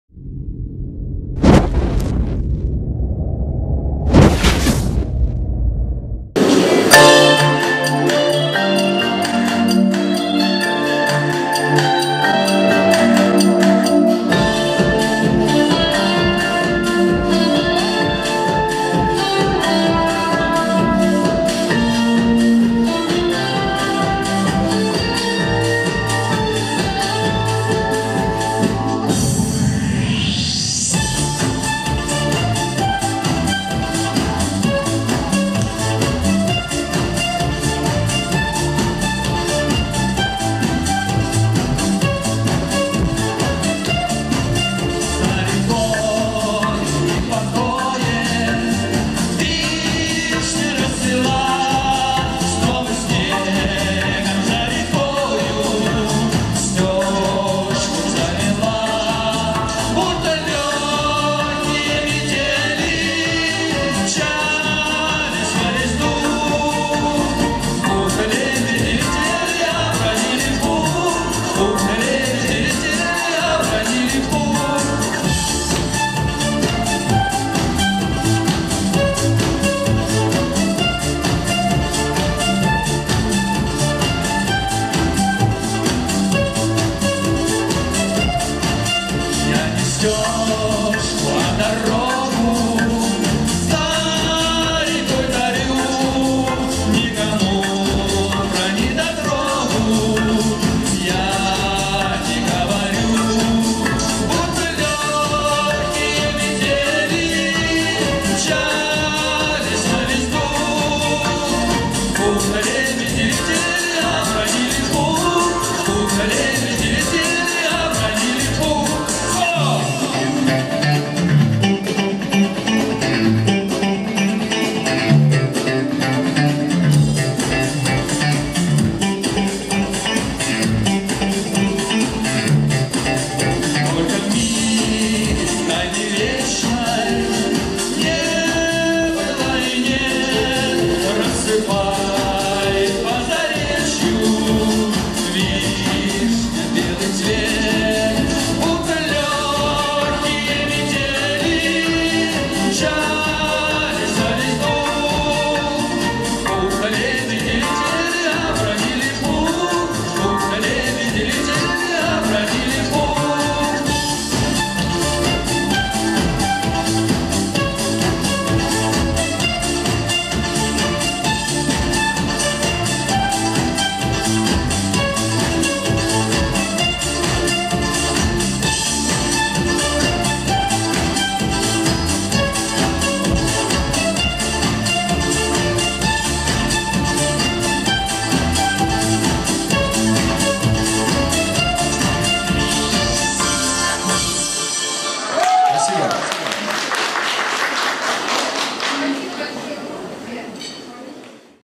Группа "Воспоминание"
Выступление брутального коллектива "Воспоминание" на вечере в честь 35-ти летия рок группы "Школьные годы" в г. Сергиев Посад, ресторан "Дубрава Холл"